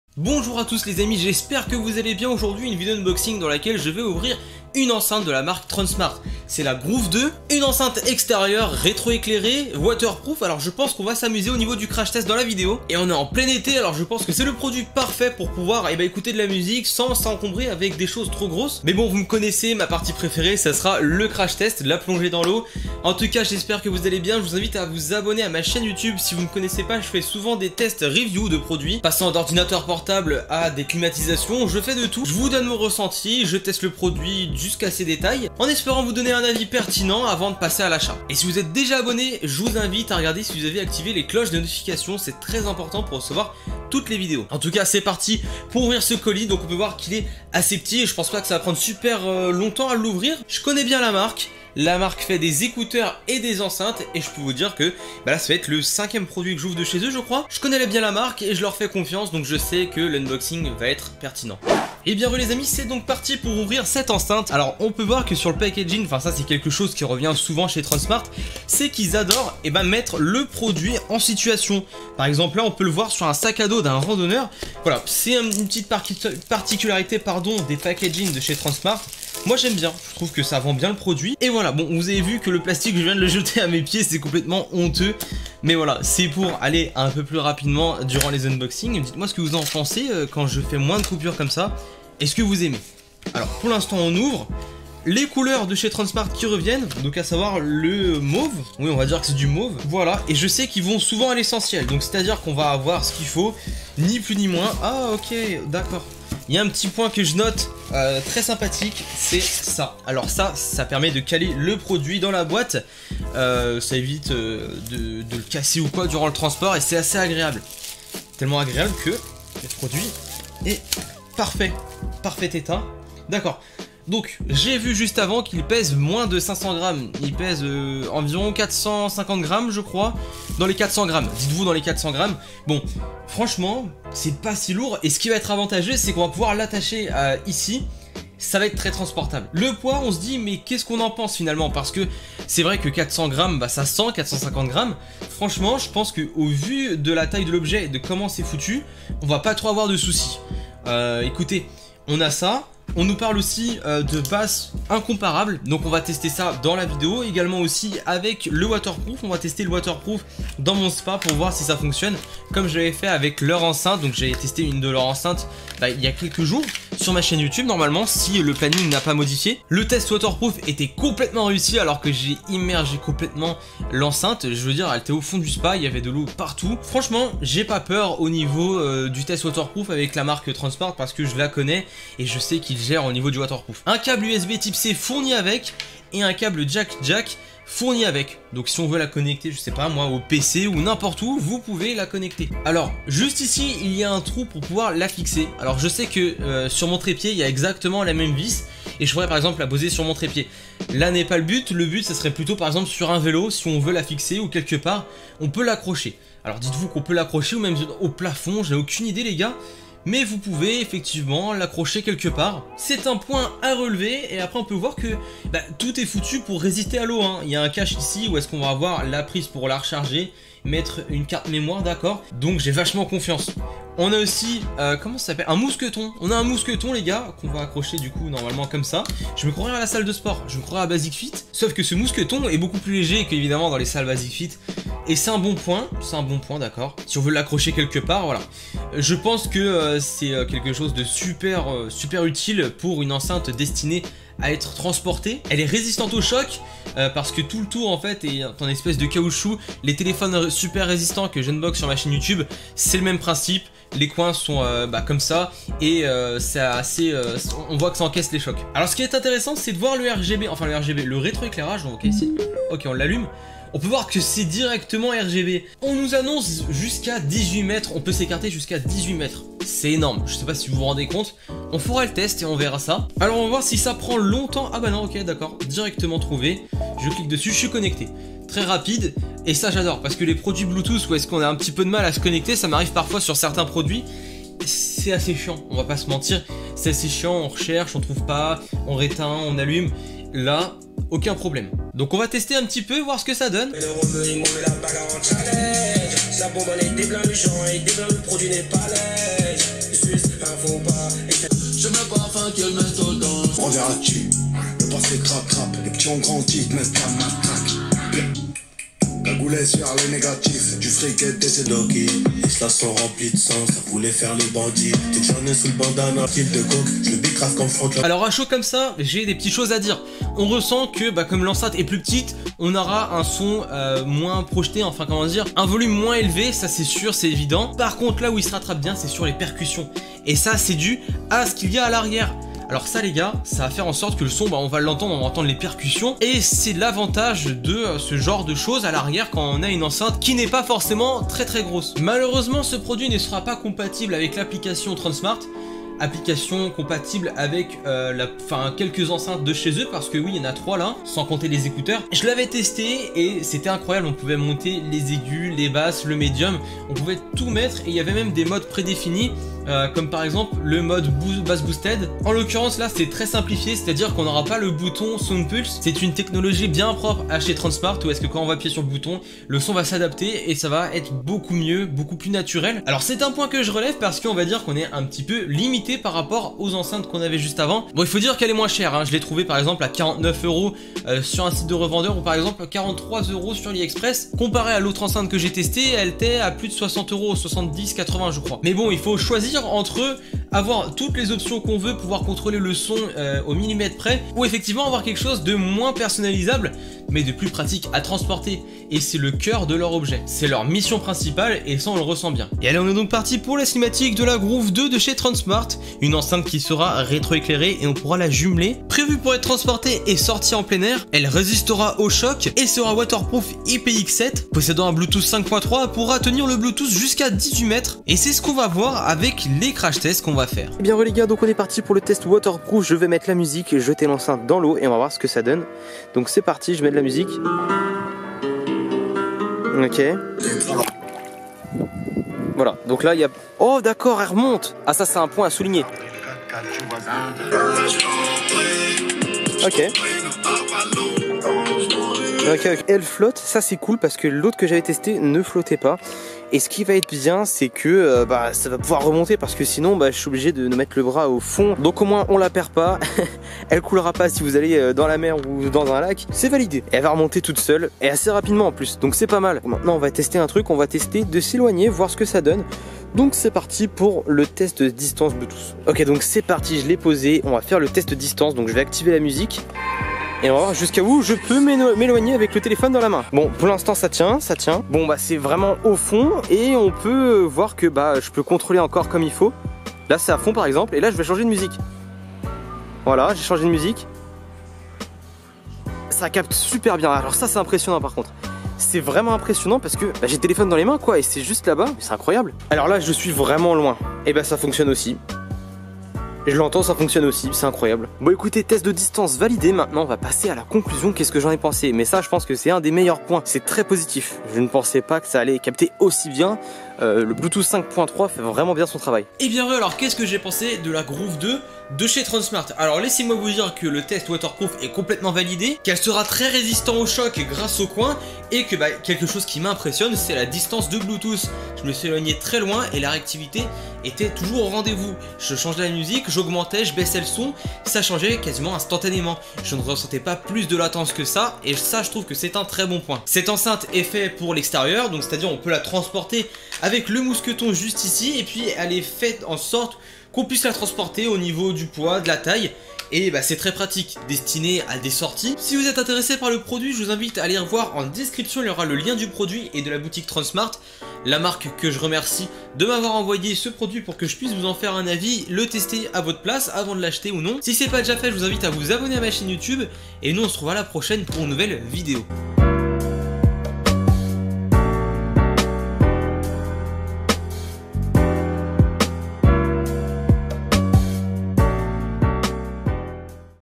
UNE MINI ENCEINTE WATERPROOF INCROYABLE ! Tronsmart Groove 2
Bonjour à tous, j'espère que vous allez bien ! Aujourd'hui on va tester l'enceinte de chez Tronsmart, la Groove 2 qui est transportable et waterproof.